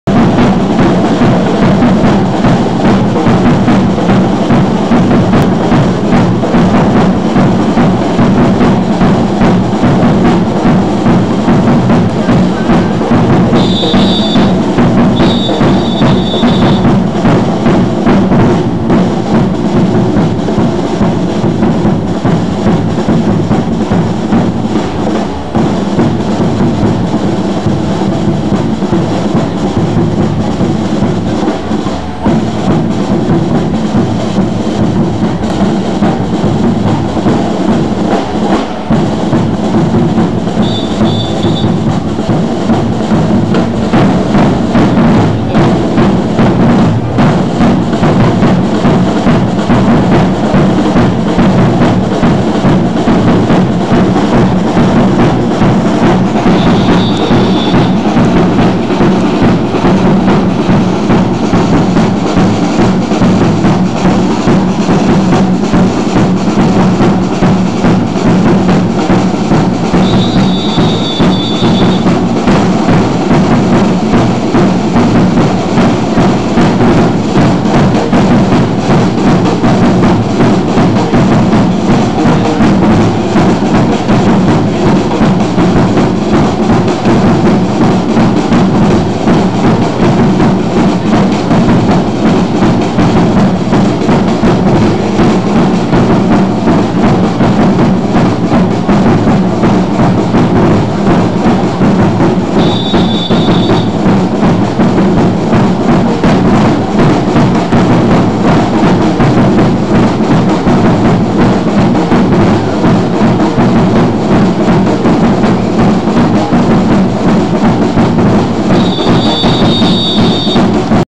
fiesta religiosa virgen de pompella 2008 la serena/ baile mapuche
la presentacion del baile religioso mapuche ante la imagen de la virgen del rosario de pompella parroquia santo domingo